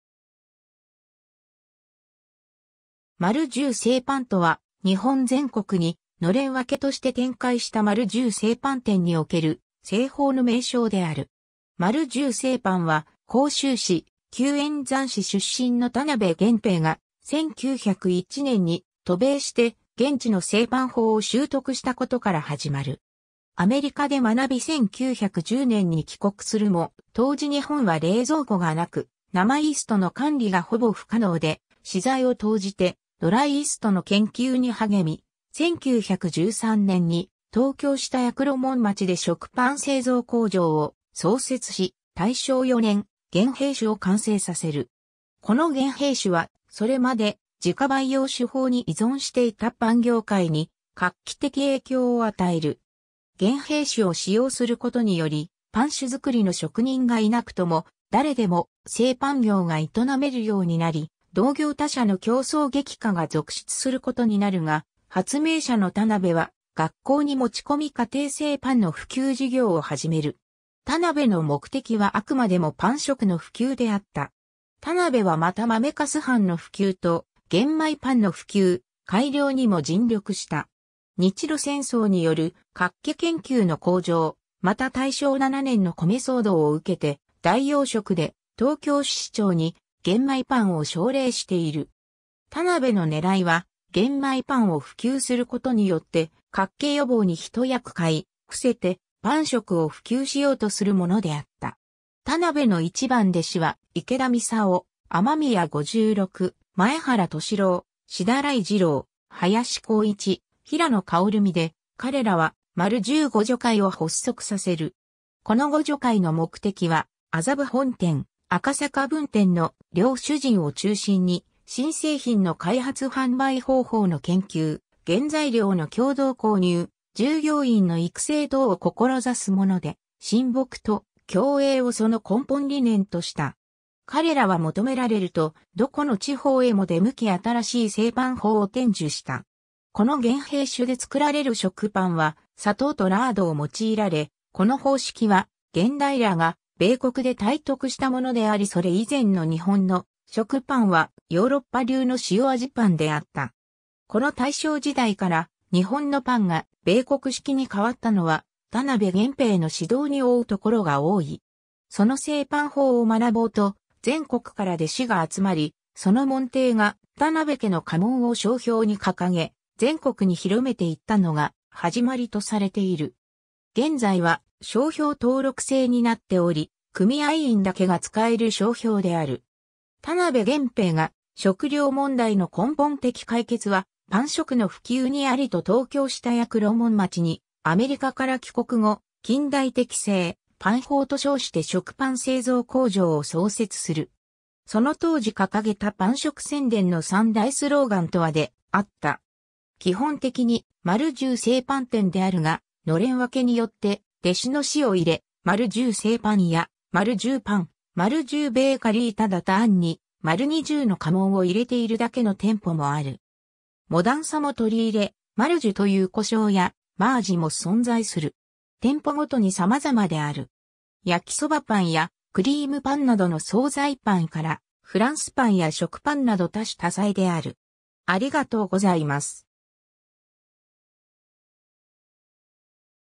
丸十製パン
丸十製パン（まるじゅうせいパン）とは、日本全国に暖簾分けとして展開した丸十製パン店における製法の名称である。
 丸十製パンは、甲州市(旧塩山市）出身の田辺玄平（1874年11月16日 - 1933年10月16日）が、1901年（明治34年）に渡米して現地の製パン法を習得したことから始まる。
 アメリカで学び1910年（明治43年）に帰国するも当時日本は冷蔵庫が無く、生イーストの管理がほぼ不可能で、私財を投じてドライイーストの研究に励み、1913年（大正2年）に東京下谷黒門町で食パン製造工場を創設し、大正4年「玄平種」を完成させる。
この玄平種は、それまで自家培養種法に依存していたパン業界に画期的影響を与える。
玄平種を使用することにより、パン種作りの職人がいなくとも誰でも製パン業が営めるようになり、同業他社の競争激化が続出することになるが、発明者の田辺は、学校に持ち込み家庭製パンの普及事業を始める。
田辺の目的はあくまでもパン食の普及であった。
 田辺はまた豆粕飯の普及と玄米パンの普及・改良にも尽力した（米騒動時に東京市長の田尻稲次郎に豆粕食の普及を進言した）。
日露戦争による脚気研究（鈴木梅太郎による「白米の食品としての価値並に動物の脚気様疾病に関する研究」）の向上、また大正7年の米騒動を受けて、代用食で東京市市長に玄米パンを奨励している。
田辺の狙いは玄米パンを普及することによって脚気予防に一役買い、伏せてパン食を普及しようとするものであった（日本の脚気史 参照）。
 田辺の一番弟子は池田操・雨宮五六・前原登志郎・志田頼次郎（東京）、林幸一（横浜）、平野芳身（浜松）で、彼らは「丸十互助会」を発足させる。
この互助会の目的は、麻布本店・赤坂分店の両主人を中心に、新製品の開発販売方法の研究、原材料の共同購入、従業員の育成等を志すもので、「親睦と共栄」をその根本理念とした。
彼らは求められるとどこの地方へも出向き新しい製パン法を伝授した。
この玄平種で造られる食パンは砂糖とラードを用いられ、この方式は玄平が米国で体得したものでありそれ以前の日本の食パンはヨーロッパ流の塩味パンであった。
この大正時代から日本のパンが米国式に変わったのは田辺玄平の指導に負うところが多い。
 その製パン法を学ぼうと、全国から弟子が集まり、その門弟が田辺家の家紋（「○」の中に「十」）を商標に掲げ、全国に広めていったのが始まりとされている。
現在は商標登録制になっており、組合員だけが使える商標である。
 田辺玄平が「食糧問題の根本的解決はパン食の普及にあり」と東京下谷黒門町にアメリカから帰国後、近代的製パン法と称して食パン製造工場を創設する。
その当時かかげたパン食宣伝の三大スローガンとは であった。
 基本的に「丸十製パン店」であるが、暖簾分けによって弟子の氏を入れ「○○丸十製パン」や「丸十○○パン」、「丸十ベーカリー」ただ単に「○に十」の家紋を入れているだけの店舗もある。
モダンさも取り入れ「マルジュ」という呼称や、「Marujyu」も存在する。
 店舗ごとにさまざまである。
 焼きそばパンやクリームパンなどの惣菜パンからフランスパンや食パンなど多種多彩である。